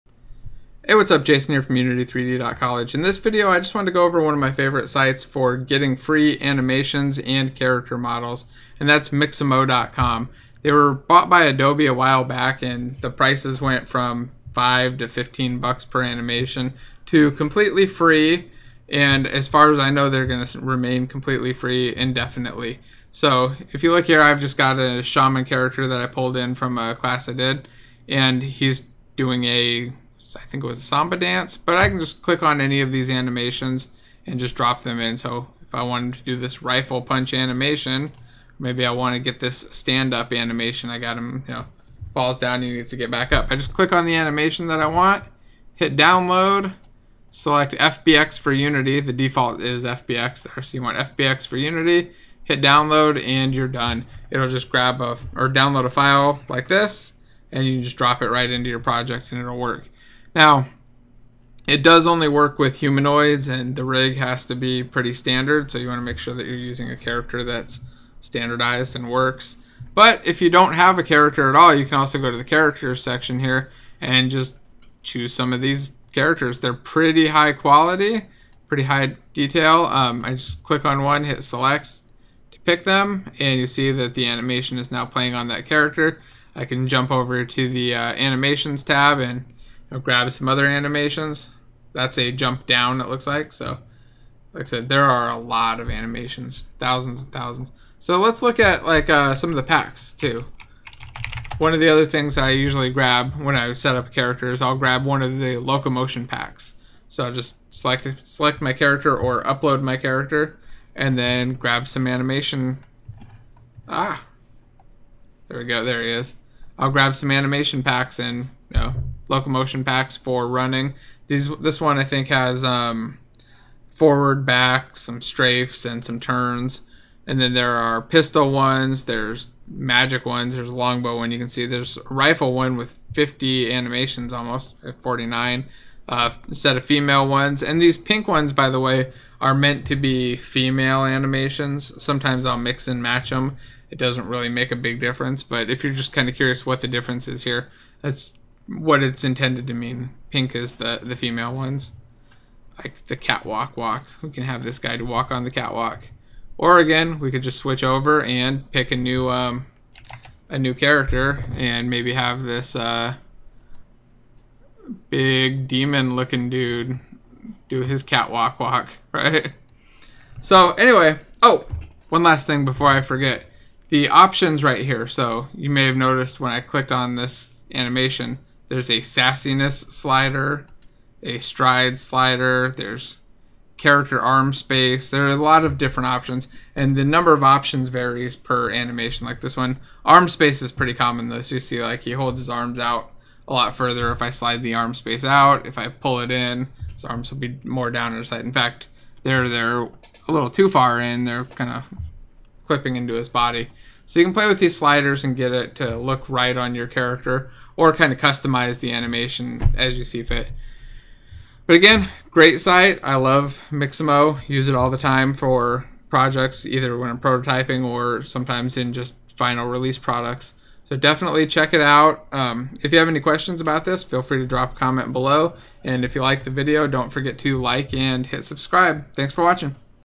My favorite place for free animations & characters
A quick rundown of my favorite site for free animations and character models.  There are 1000's of animations waiting to be added to your game freely! :)